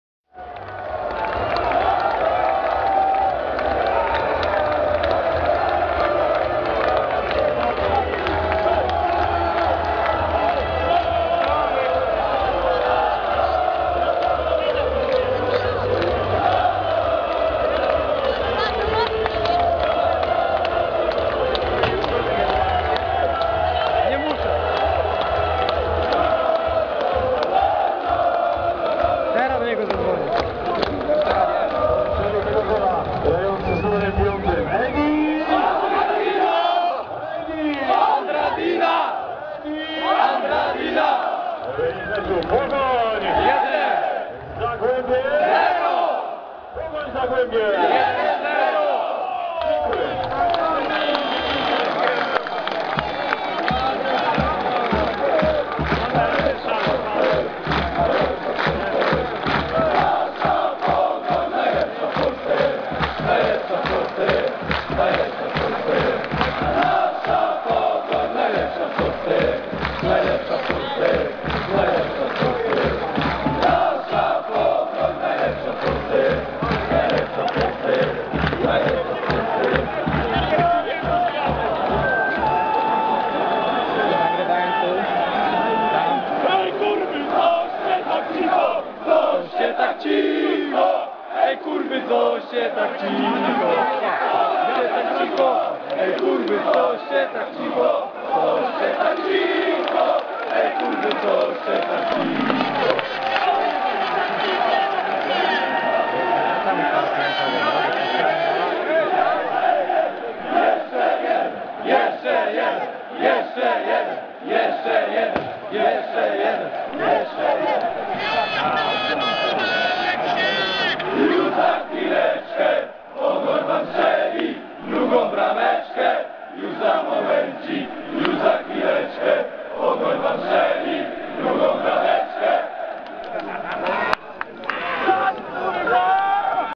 Pogoń Szczecin - Zagłębie Lubin 4:0 - bramka na 1:0 - Edi Andradina
PIerwszy gol w Ekstraklasie sezonu 2012/2013, a jej autorem Edi Andradina, który precyzyjnym strzałem z rzutu wolnego pokonał bramkarza Zagłębia Lubin.